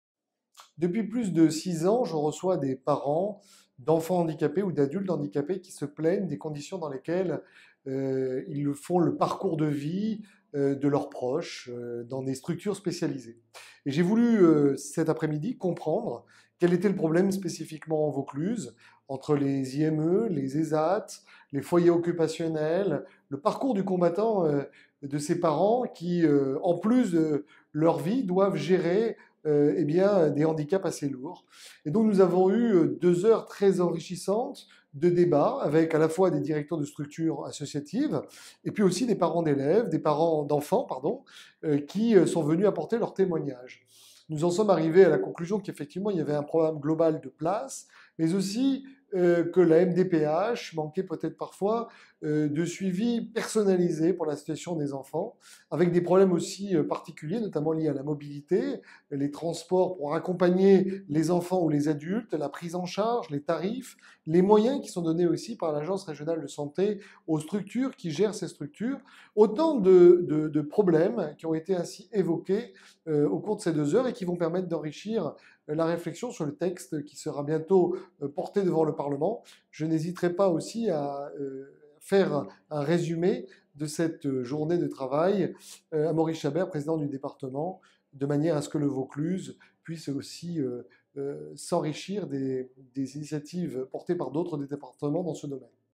Réunion autour du l'handicap
La solidarité est une valeur qui nous rassemble tous.

J’ai organisé une rencontre autour du handicap en Vaucluse, à ma permanence de Carpentras.

La solidarité est une composante essentielle du Pacte républicain qui permet à chacun d'entre nous de pouvoir se réaliser quelques soient nos différences.

J'ai réuni tous les acteurs : responsables associatifs, structures d'accueil, foyers occupationnels ou de loisir de tout le Département, CAF et parents d’enfants ou d’adultes en situation de handicap. Un tour d’horizon à 360 degrés du sujet !